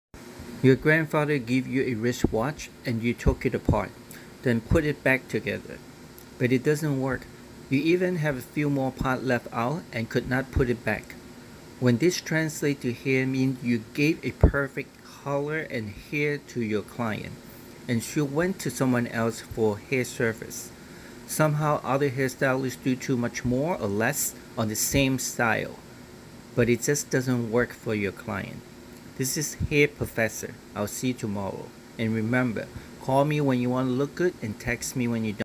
#357 Your grandfather give you a wrist watch and you took it apart then put
Your grandfather give you a wrist watch and you took it apart then put it back together, but it doesn't work, you even had few more part left out and could not put it back, when this translate to hair mean you gave a perfect haircut and color to your clients and she went to someone else for the hair service, somehow other hairstylist do too much more or less on the same hairstyle, but it just doesn't work for your clients.